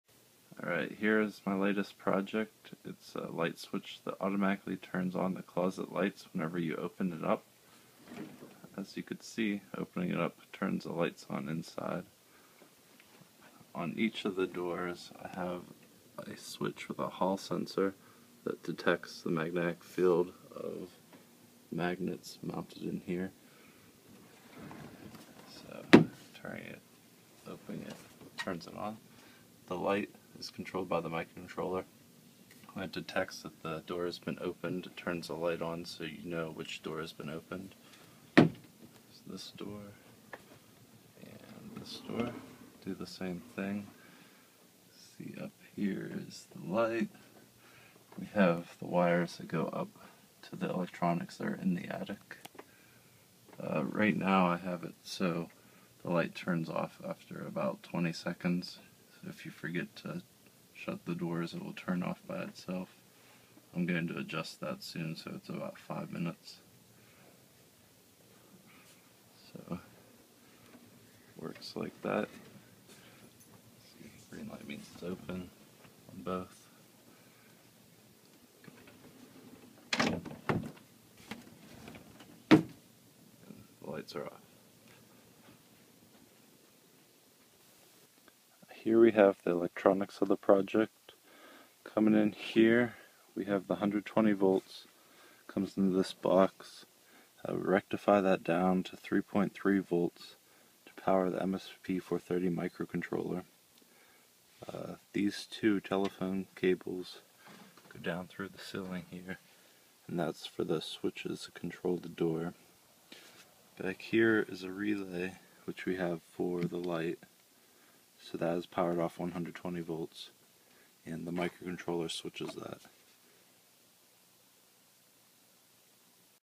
Closet Door Lightswitch
A project that detects when a closet door is opened and then automatically turns on the closet lighting. It is controlled by a TI MSP430G2001 microcontroller that is powered from 3.3V DC derived from 120V AC line voltage. Reed switches and magnets on the doors of the closet act as a switch so the microcontroller can detect when the door is opened and turn on an LED indicating that one of the doors is open. If at least one of the doors is open, the microcontroller switches a relay to turn on an overhead light. The light is automatically shut off after five minutes if the doors are not shut.